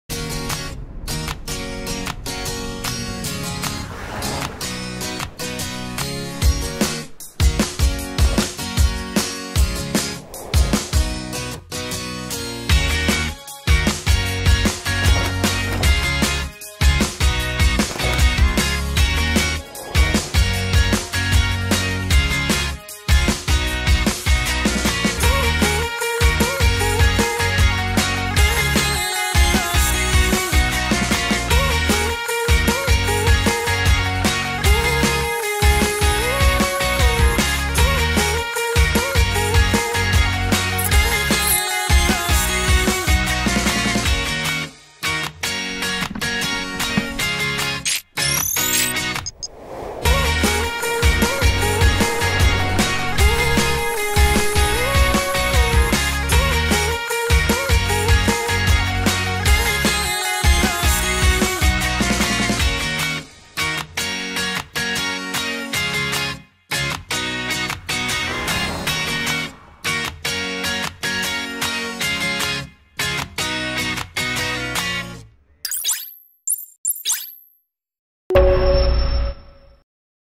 Yamaha NEO's: Move Smart while travelling | Wheel Hunter 2024
Travelling in a motorhome gives you the freedom you want, without limits, without schedules. But what happens when you reach your destination? The Yamaha NEO’s electric scooter is your ideal complement. Move Smart in an agile way, without noise or fumes and expand your experience. Discover it!

Viajar en autocaravana te da una libertad que solo tú sabes, sin límites, sin horarios ¡A tu aire! Pero, ¿qué pasa cuando llegas a tu destino? La nueva YAMAHA eléctrica NEO’S es tu complemento ideal. Muévete con inteligencia de forma ágil, sin ruido ni humos y amplia tus vivencias. ¡Descúbrela!